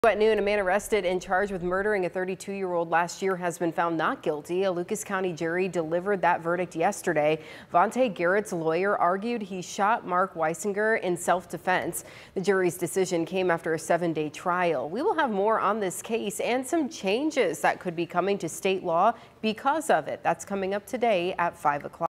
Jury finds man not guilty on three charges in 2022 south Toledo homicide
Vontae Garrett was found not guilty of murder, felonious assault and a firearm charge in the 2022 homicide of Mark Wysinger.